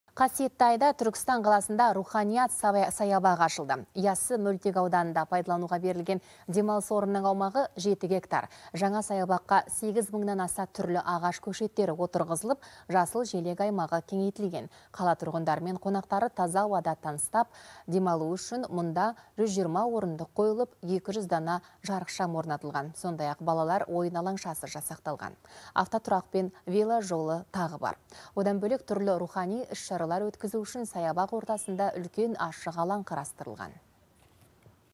Қасиетті айда Түркістанда «Руханият» саябағы ашылды
Яссы мөлтек ауданында пайдалануға берілген демалыс орнының аумағы – 7 гектар. Жаңа саябаққа 8 мыңнан аса түрлі ағаш көшеттері отырғызылып, жасыл-желек аймағы кеңейтілген. Қала тұрғындары мен қонақтары таза ауада тыныстап, демалуы үшін мұнда 120 орындық қойылып, 200 дана жарықшам орнатылған.